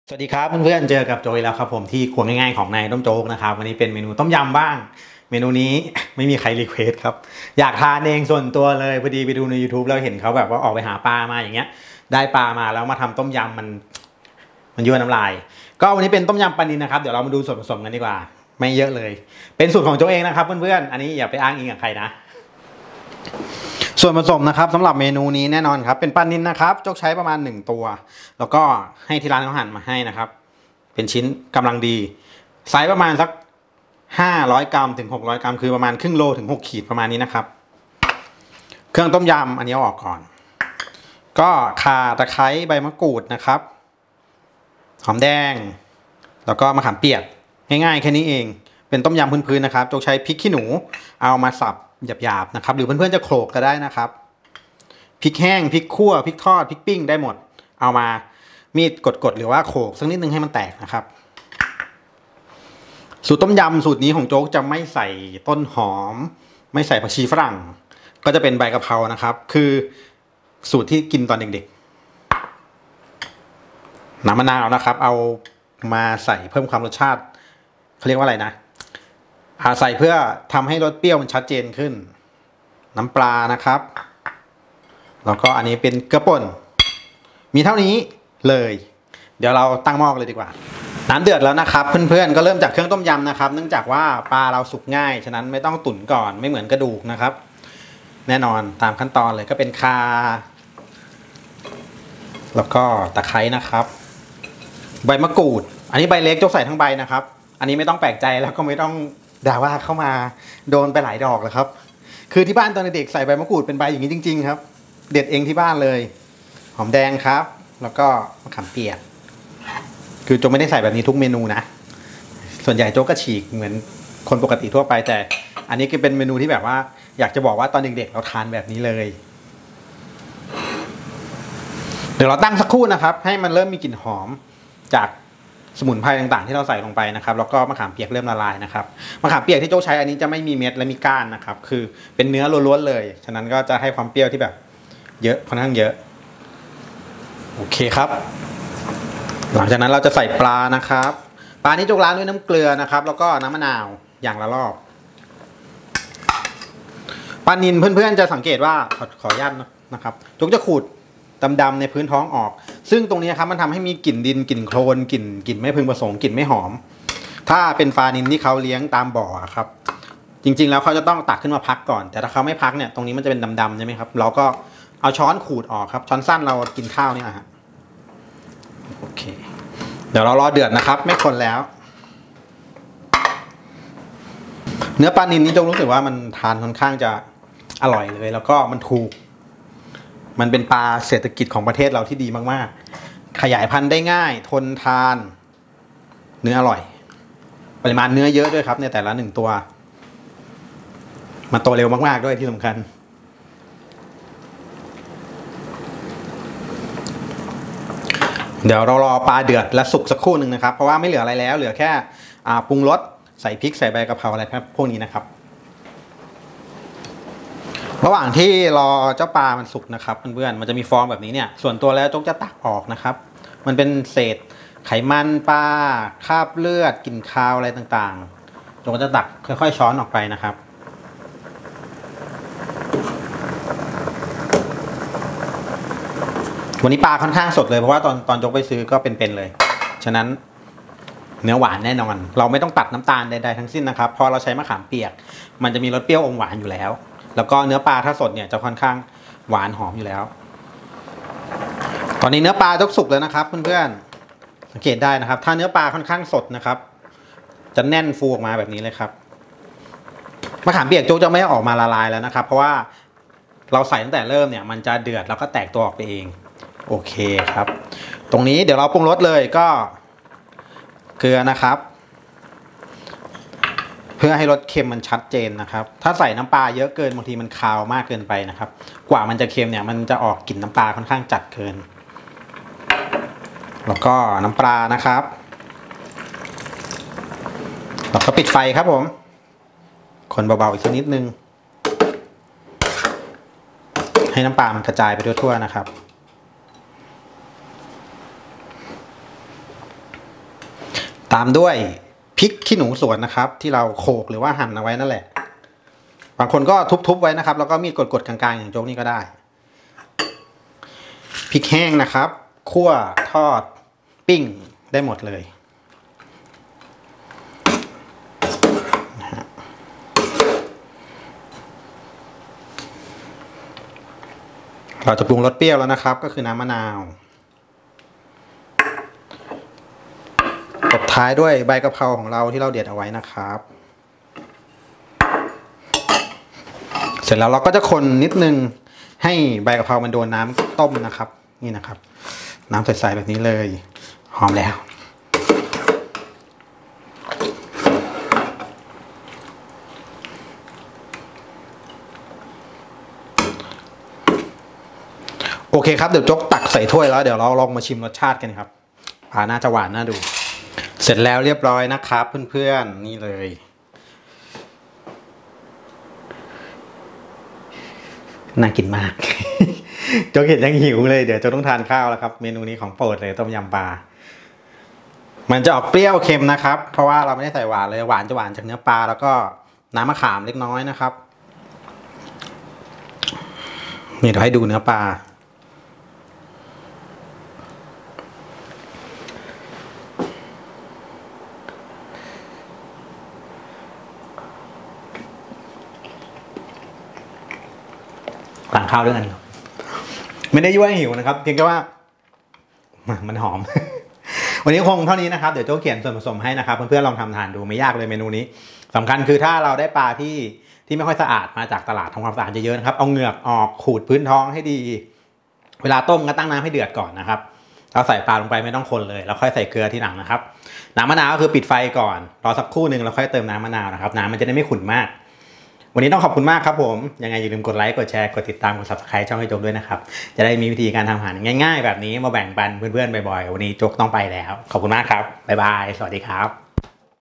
ต้มยำปลานิล สูตรนายต้มโจ๊ก อร่อยหอมไม่คาว/Fish soup recipe
วิธีทำต้มยำปลานิลสูตรนายต้มโจ๊กครับ อร่อยหอมไม่คาวใส่ใบกะเพราแซ่ยมาก

ปลานิล  500 กรัม
ข่า 5 ชิ้น
ตะไคร้ 3 ต้น
มะขามเปียก 1 ช้อนโต๊ะ
พริกขี้หนูสวน 1 ถ้วย
พริกแห้ง 1 ถ้วย
ใบกะเพรา 1 ถ้วย

Ingredients
500g  Tilapia
5 Galanga
3 Lemon Grass
6Kaffir Lime Leaves
2 Shallot
1tbsp Tamarind
1Cup Bird's eye chili
1Cup Dried chillies
1Cup  Holy Basil
3tbsp  Lime Juice
2tbsp Fish sauce
½ tbsp. Salt

พูดคุยกันที่แฟนเพจนายต้มโจ๊ก